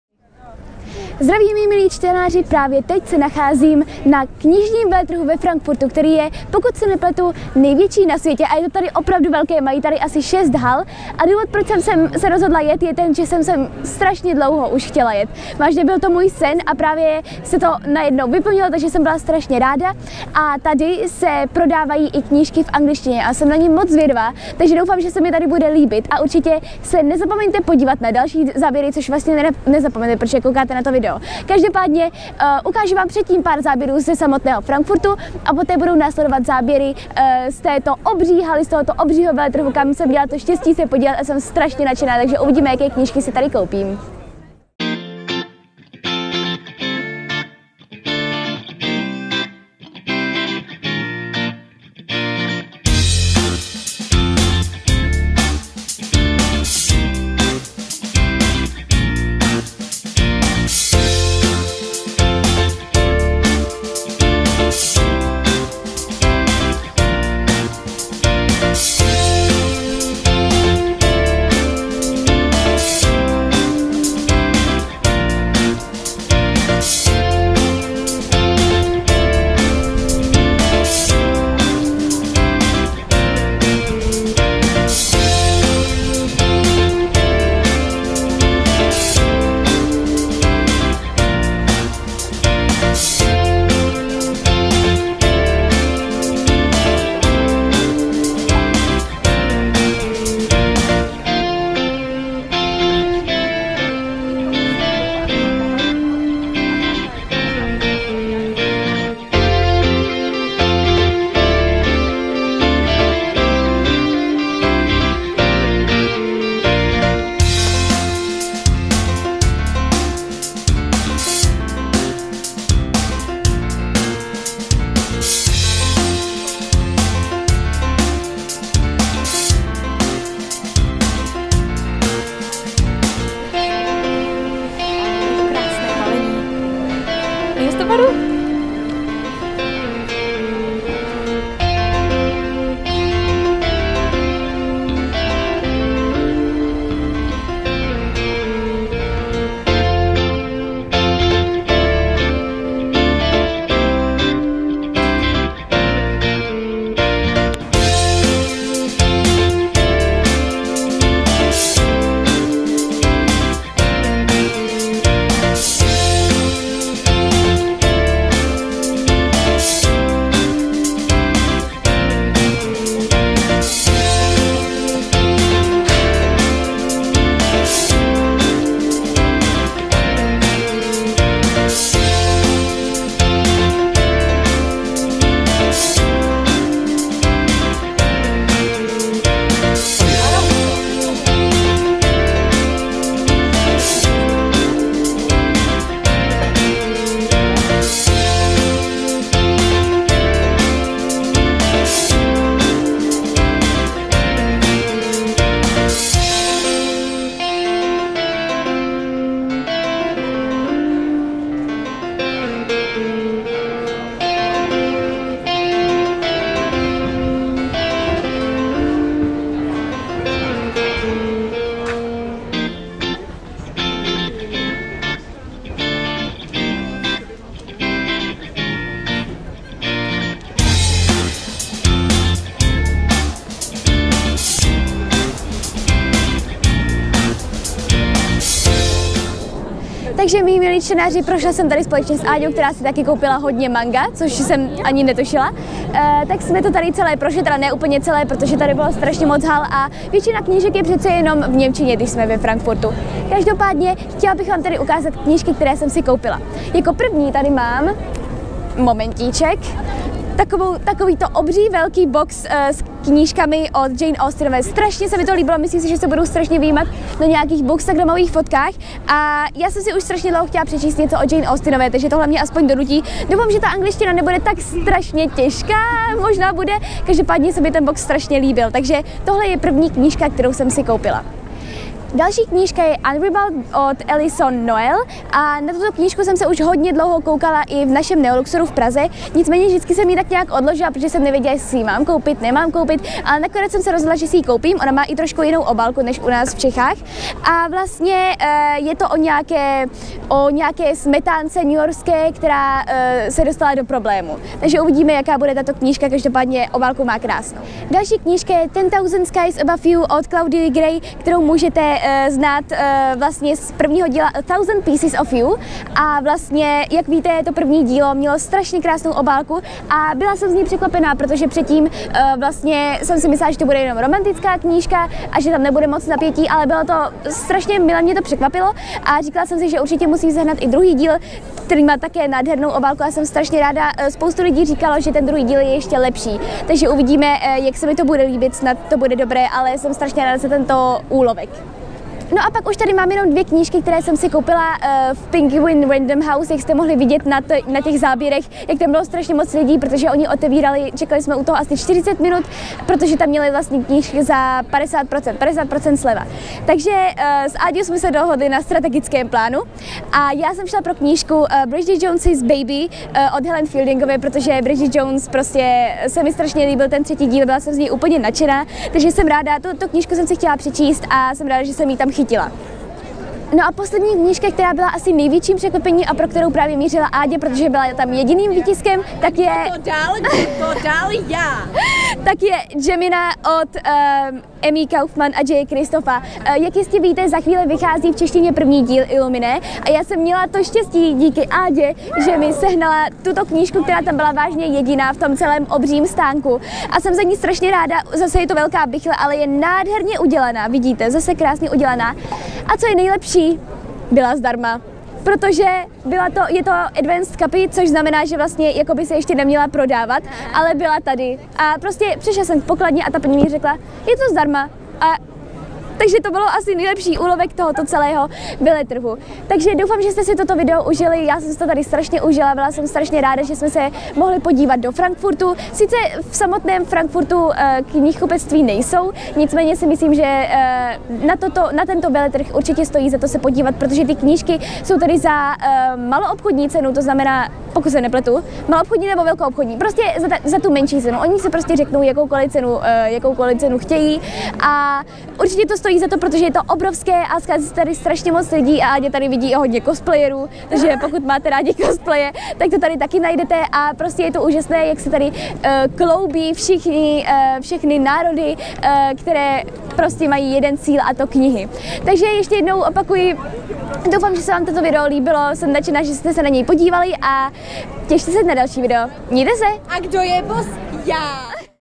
Klářiny knihy - Frankfurtský knižní veletrh
Zdravím, mí milí čtenáři! SPLNIL SE MI SEN! Před týdnem jsem měla možnost se podívat na největší knižní veletrh na světě, který se koná ve Frankfurtu. Podívejte se na to, jak se mi tam líbilo! :)